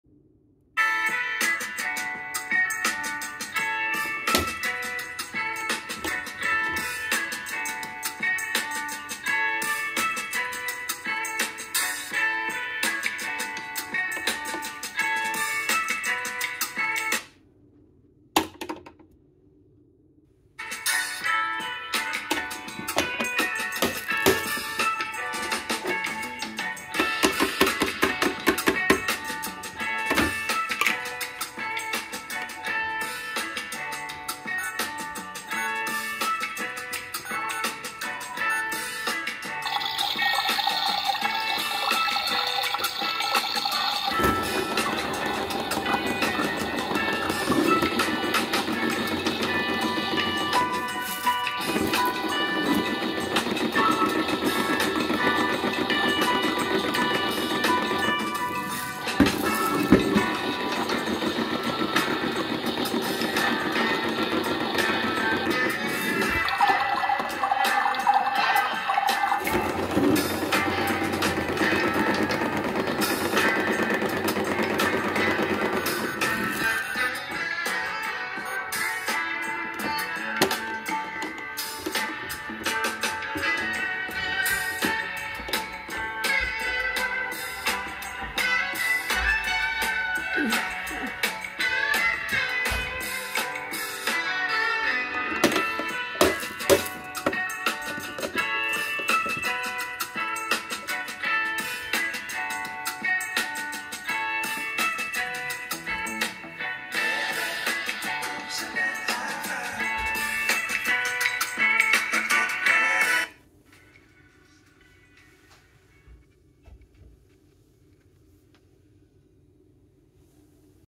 Pipe Bandage On Hand Wash In Kitchen Toy Washing machine. (Copied By The Becker Boys).
The Reason Why I Had To Tilt The Machine Backwards Is Because The Drum Slowed Down Slightly And When I Tilted It Backwards The Drum Speeded Up.